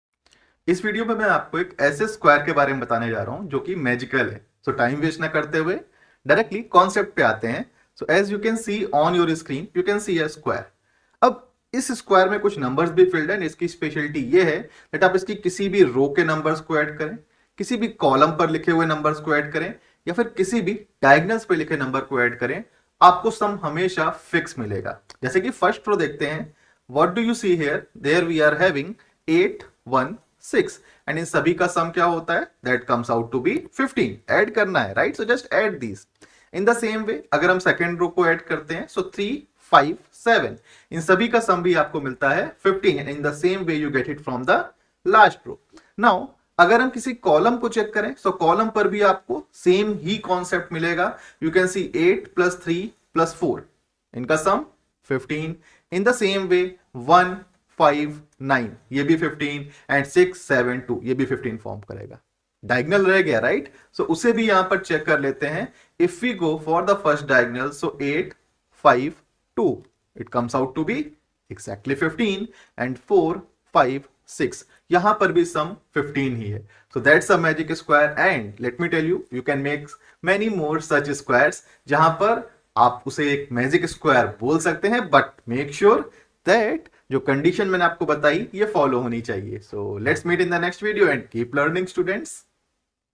Magic Square - untold math concepts
Here you get to know about magic Square, let me know your thoughts in comments